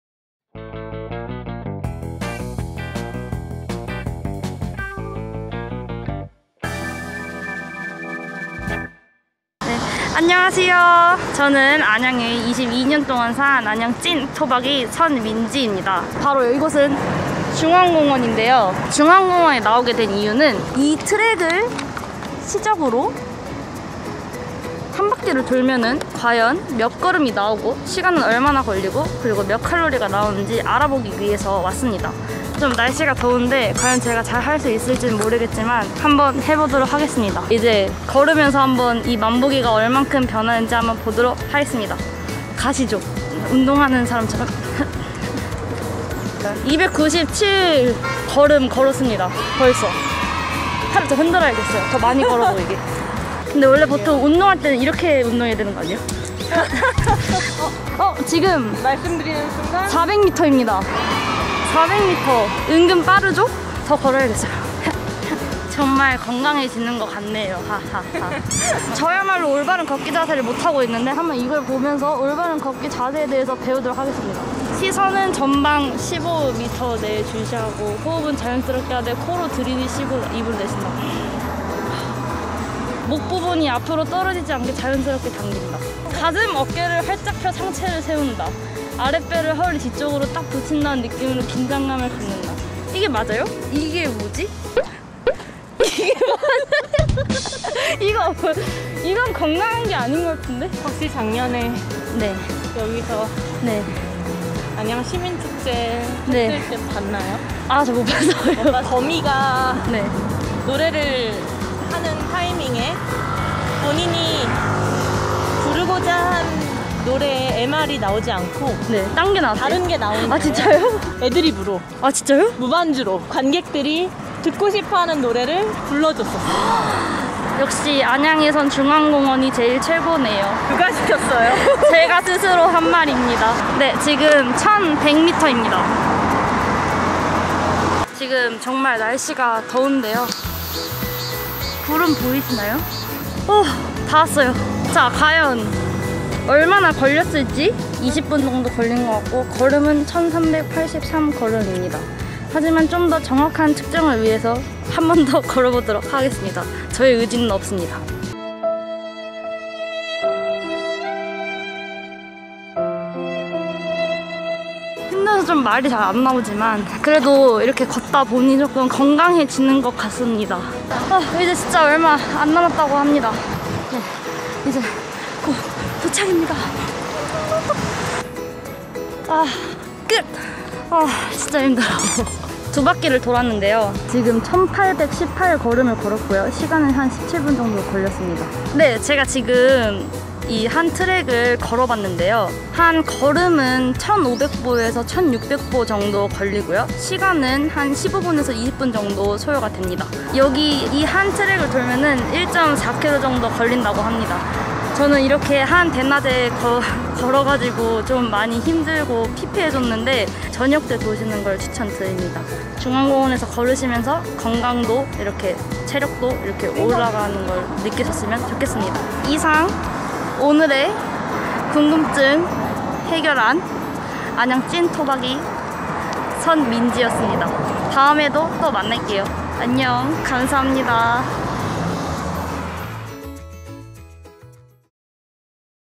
호기심안양 | 안양시 중앙공원 한바퀴 다 돌면 몇 걸음이나 되나요? 궁금증을 풀어드립니다!
안양 찐토박이가 안양시 곳곳 궁금증을 해소해드립니다!

💡두번째 호기심,
안양시 중앙공원 , 한바퀴 다 돌면 몇 걸음이나 걷게 되는걸까?
찐토박이가 한보 한보 직접 걸어봤습니다🚶‍♀️
한 바퀴 당 걸리는 시간과 거리는?!
※해당 영상은 코로나19 철저한 방역 아래 진행되었습니다.